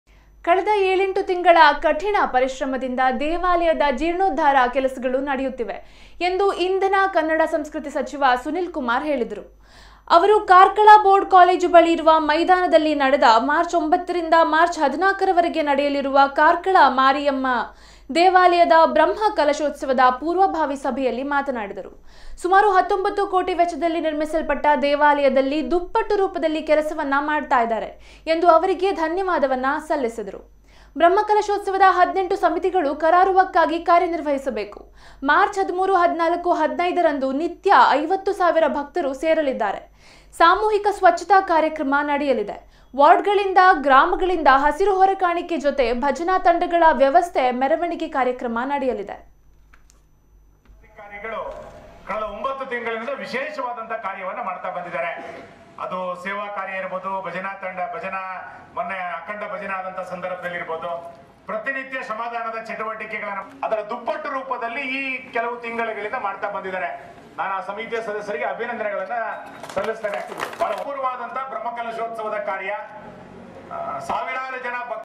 ಕಾರ್ಕಳದ ಮಾರಿಯಮ್ಮ ದೇವಾಲಯದ ಬ್ರಹ್ಮಕಲಶೋತ್ಸವದ ಪೂರ್ವಭಾವಿ ಸಭೆ
ಕಳೆದ ಏಳೆಂಟು ತಿಂಗಳ ಕಠಿಣ ಪರಿಶ್ರಮದಿಂದ ದೇವಾಲಯದ ಜೀರ್ಣೋದ್ಧಾರ ಕೆಲಸಗಳು ನಡೆಯುತ್ತಿವೆ ಎಂದು ಇಂಧನ ಕನ್ನಡ ಸಂಸ್ಕöÈತಿ ಸಚಿವ ಸುನೀಲ್ ಕುಮಾರ್ ಹೇಳಿದರು. ಅವರು ಕಾರ್ಕಳ ಬೋರ್ಡ್ ಕಾಲೇಜು ಬಳಿ ಇರುವ ಮೈದಾನದಲ್ಲಿ ನಡೆದ ಮಾ.9 ರಿಂದ ಮಾ.14 ರವರೆಗೆ ನಡೆಯಲಿರುವ ಕಾರ್ಕಳದ ಮಾರಿಯಮ್ಮ ದೇವಾಲಯದ ಬ್ರಹ್ಮಕಲಶೋತ್ಸವದ ಪೂರ್ವಭಾವಿ ಸಭೆಯಲ್ಲಿ ಮಾತನಾಡಿದರು.